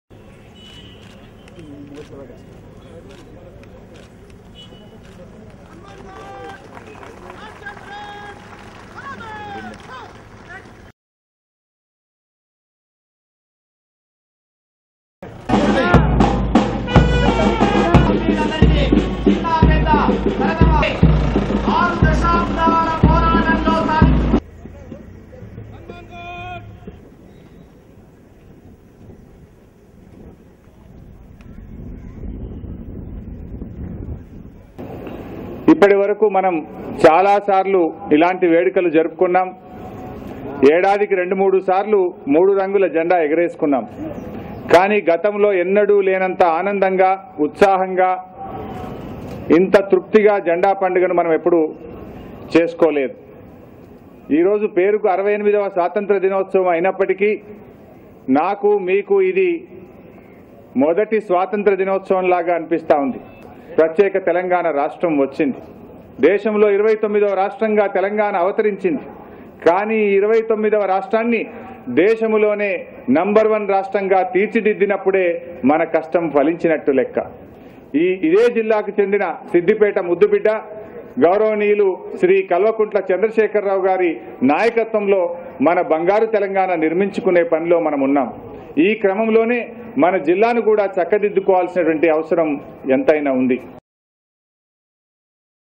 Telangana Irrigation Minister Harish Rao hoisting Indian National Flag in Medak
Watch TRS leader and Minister for Irrigation, Marketing & Legislative Affairs, Harish Rao hoisting the Indian National Flag on the occasion of 68th Independence Day in Medak.